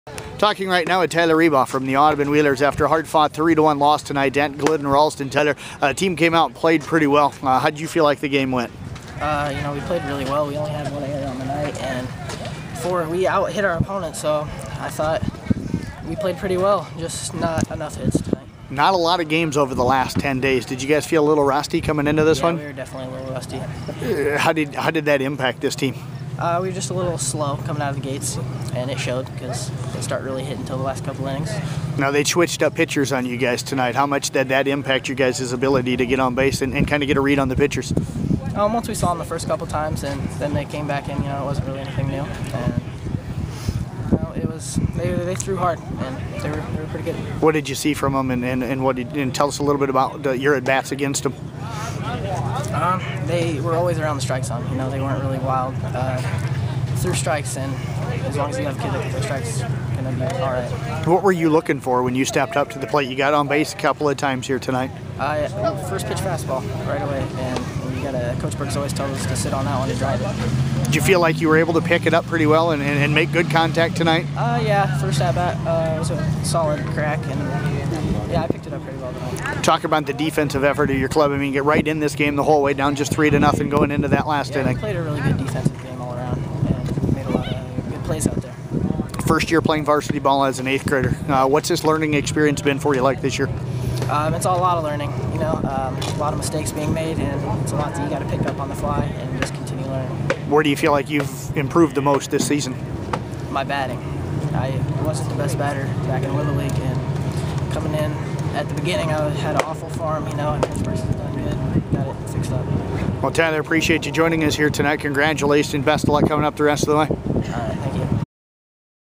Tyler Riebhoff of Audubon Vs Glidden Ralston
Riebhoff goes 1-1 with HBP and BB